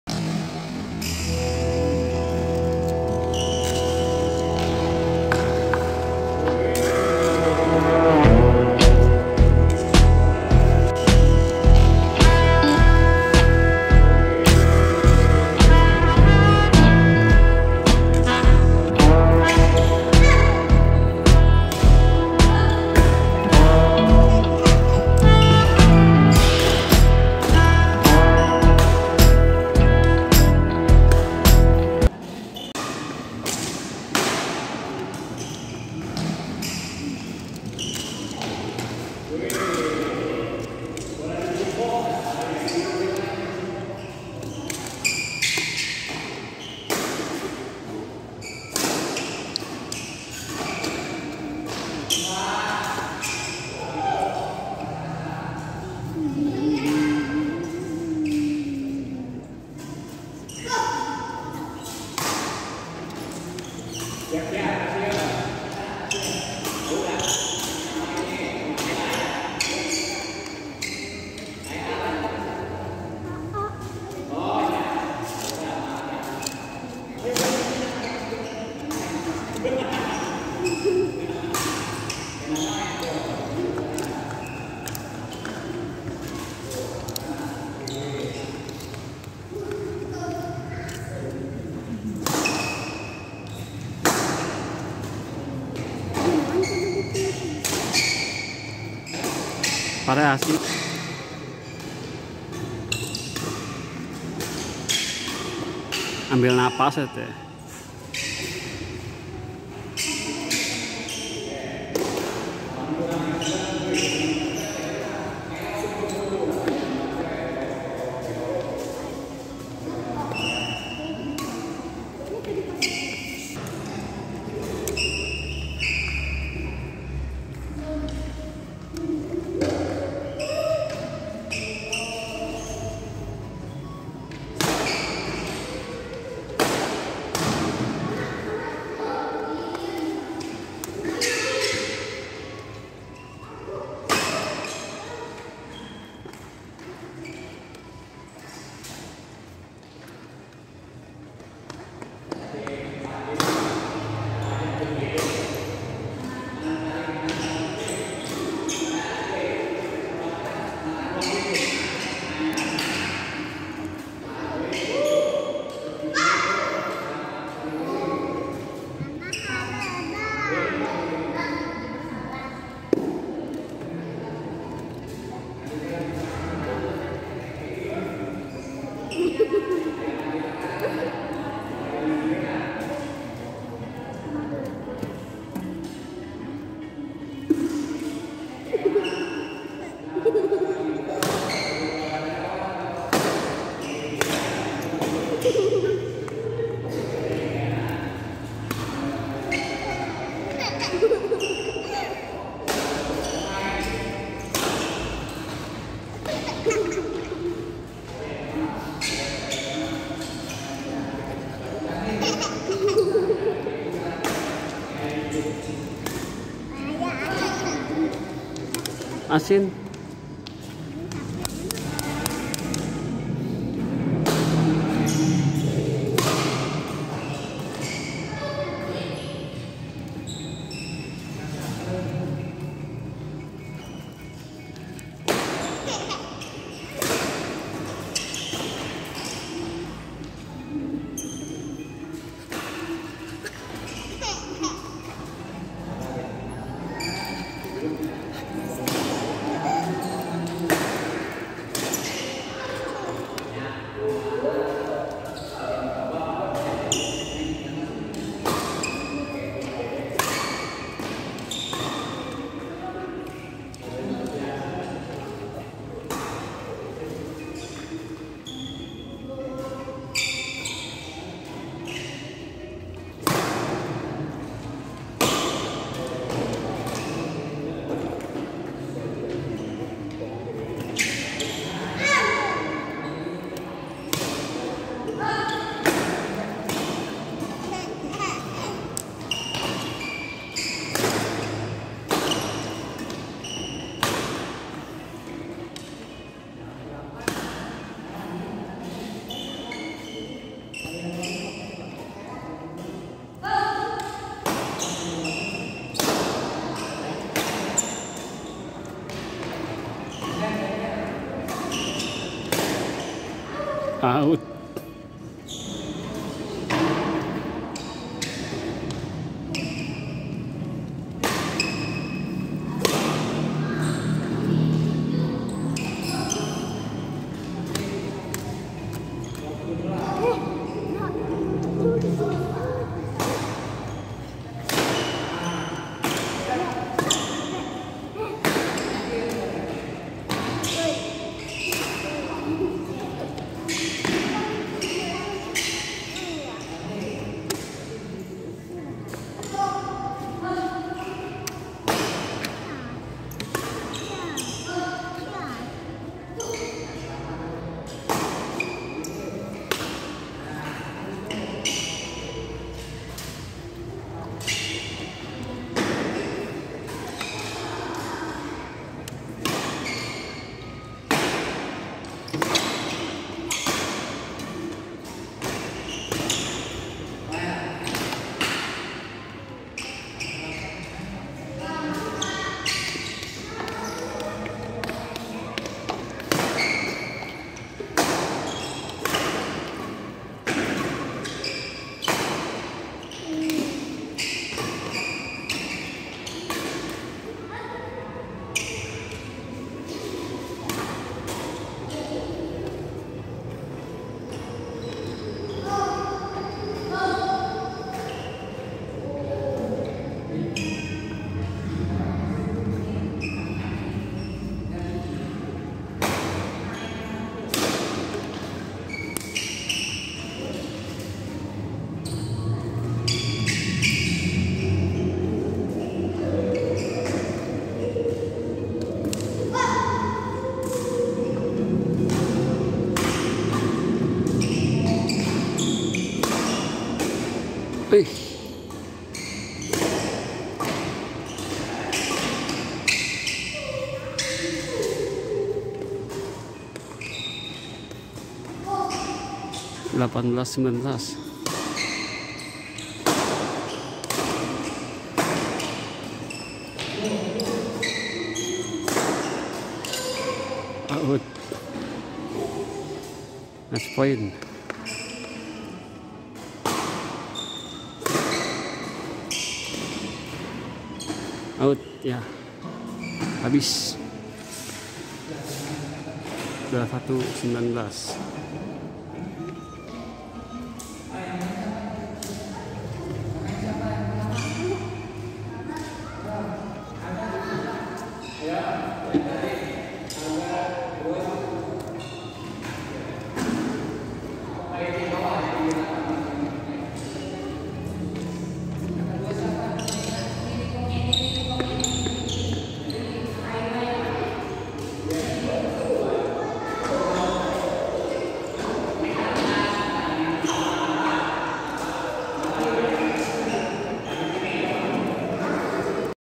Viral !! pertandingan bulutangkis bocil skill dewa juara 02sn
Viral !! Pertandingan bulutangkis bocil skill dewa juara 02sn sd  kecamatan leuwimunding, lagi latihan di pb galuh antara raffa juara 02sn dengan sandi